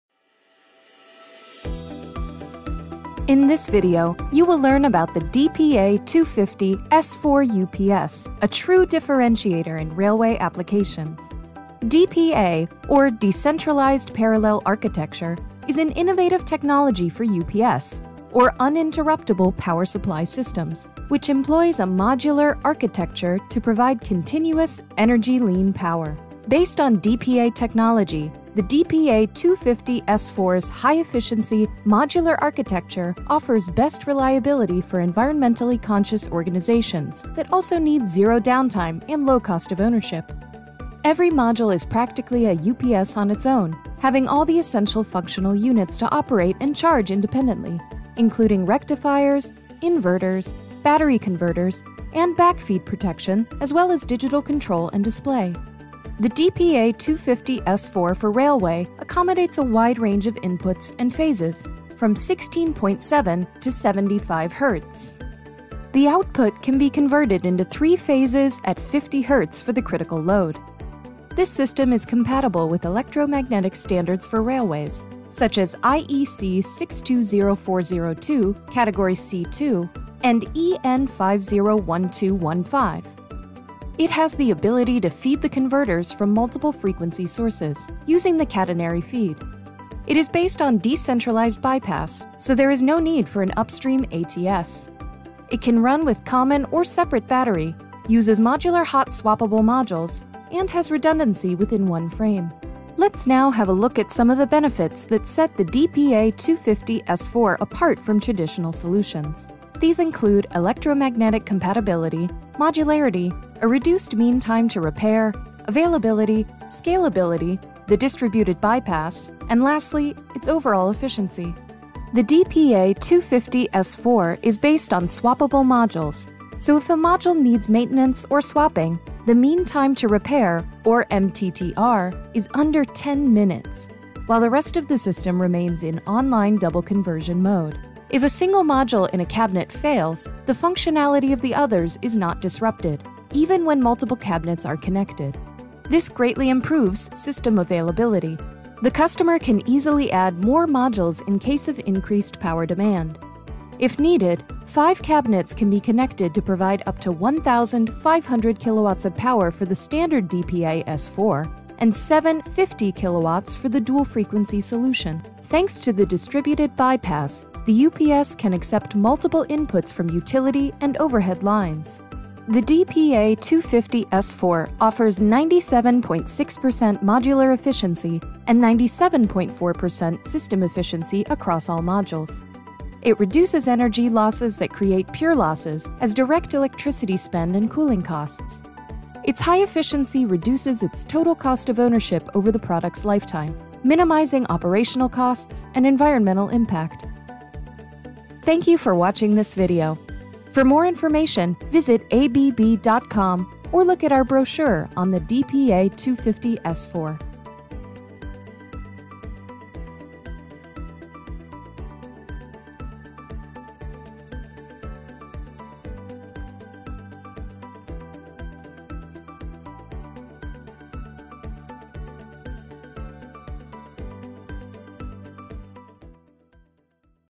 How to design power continuity solutions for railway applications?
In this video, you will learn about the DPA 250 S4 UPS, a true differentiator in railway applications. DPATM, or decentralized parallel architecture, is an innovative technology for UPS, or uninterruptible power supply systems, which employs a modular architecture to provide continuous, energy-lean power. During the video you will be presented with all DPA 250 S4  benefits.

Go directly to the various steps:
• Features – 0:33
• Flexibility – 1:28
• Benefits – 1:50
• Reduced Mean Time To Repair (MTTR) – 2:05
• Availability – 2:20
• Scalability – 2:30
• Efficiency – 2:50